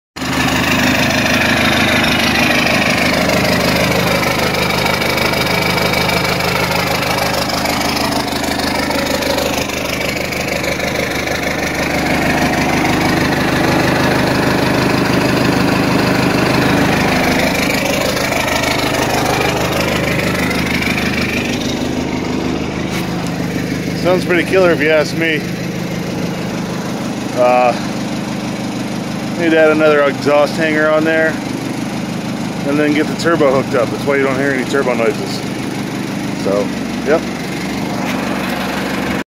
F150 4BT Swap July 2021
Just a vid of the first fire up of a 4BT in my F150.